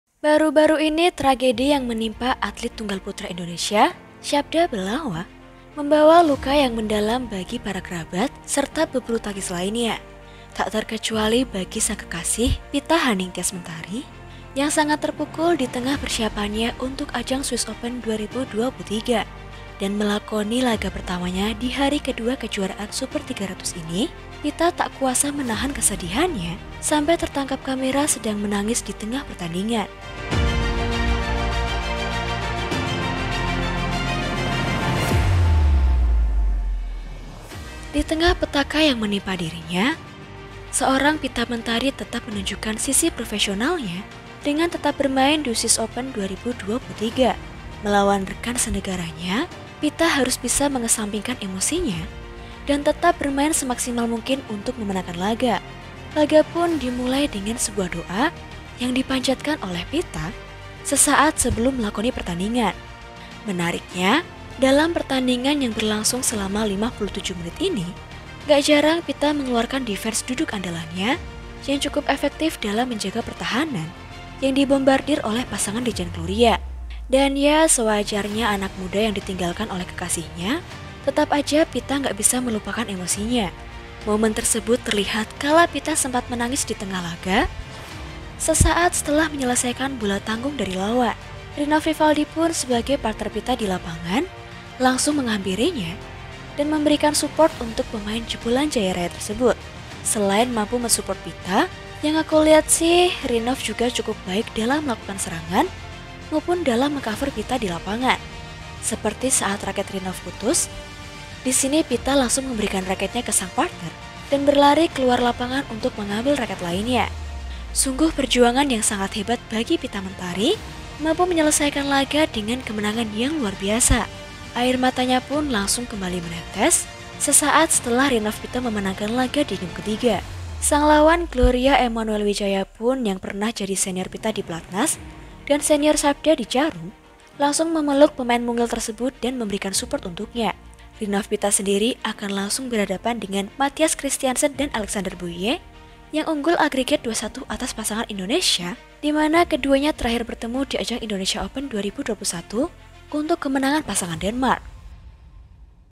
"Dipeluk KAWAN & LAWAN" 😭 Momen TANGIS EMOSIONAL Pitha Mentari Bawa Kemenangan di Swiss Open 2023 😥
Baru-baru ini tragedi yang menimpa atlet tunggal putra Indonesia Syabda Belawa, membawa luka yang mendalam bagi para kerabat, serta pebulutangkis lainnya. Tak terkecuali bagi sang kekasih Phita Haningtyas Mentari, yang sangat terpukul ditengah persiapannya untuk ajang Swiss Open 2023. Dan melakoni laga pertamanya dihari kedua kejuaraan super 300 ini, Phita tak kuasa menahan kesedihannya, sampai tertangkap kamera sedang menangis ditengah pertandingan.